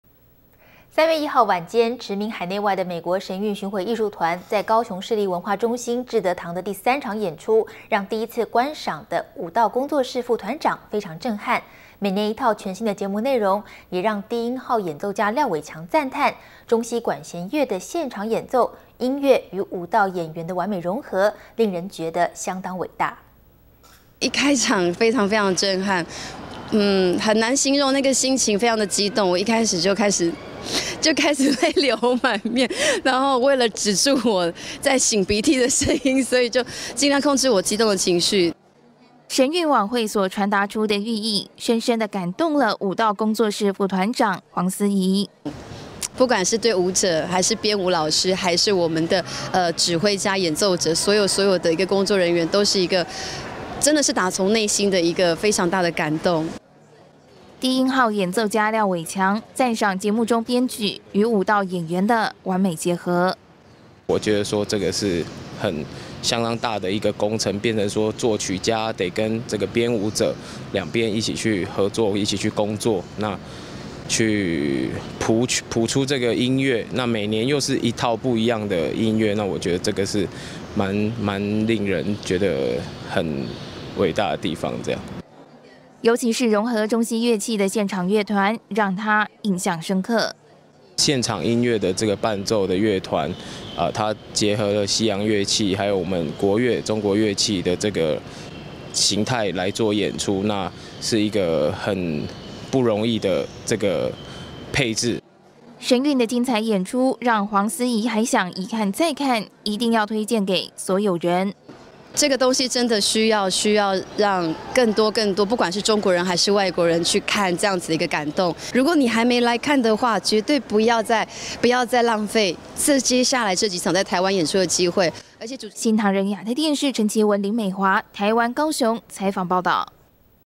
【2013神韻_舞蹈音樂人讚譽_神韻藝術團】神韻演出內涵深遠令人感動
【2013神韻_舞蹈音樂人讚譽_神韻藝術團】神韻演出內涵深遠令人感動(NTDAPTV) 3月1日夜晚，馳名海內外的美國神韻巡迴藝術團在高雄至德堂第三場演出，讓第一次觀賞的舞蹈工作室副團長非常震撼，每年一套全新的節目內容，讓低音號演奏家廖偉強讚嘆，中西管弦樂的現場演奏，音樂與舞蹈演員的完美融合，令人覺得相當偉大。【新唐人亞太台2013年3月4日訊】

舞蹈工作室副團長 黃思宜：「一開場非常非常的震撼，很難形容那個心情非常的激動，我一開始就開始淚流滿面，然後為了止住我在擤鼻涕的聲音，所以就盡量控制我激動的情緒。」

神韻晚會所傳達出的寓意深深的感動了舞蹈工作室副團長黃思宜。

舞蹈工作室副團長 黃思宜：「不管是對舞蹈演員還是編舞老師，還是我們的指揮家、演奏者，所有所有的一個工作人員，都是真的是打從內心一個非常大的感動。」

低音號演奏家廖偉強，讚賞節目中編曲與舞蹈演員的完美結合。

低音號演奏家 廖偉強：「我覺得這個是相當大的工程，變成說作曲家得跟編舞者兩邊一起合作一起工作，去譜出這個音樂，那每年又是一套不一樣的音樂，那我覺得這個是滿令人覺得很偉大的地方。」

尤其是融合中西樂器的現場樂團，讓他印象深刻。

低音號演奏家 廖偉強：「現場音樂的伴奏樂團，結合了西洋樂器還有中國樂器的型態來做演出，是一個很不容易的配置。」

神韻的精彩演出讓黃思宜還想一看再看，一定要推薦給所有人。

舞蹈工作室副團長 黃思宜：「這個東西（神韻）真的需要讓更多更多，不管是中國人還是外國人去看這樣子的一個感動，如果你還沒來看的話，絕對不要再，不要再浪費，接下來這幾場在台灣演出的機會。」新唐人亞太電視 陳麒文 林美華 台灣高雄採訪報導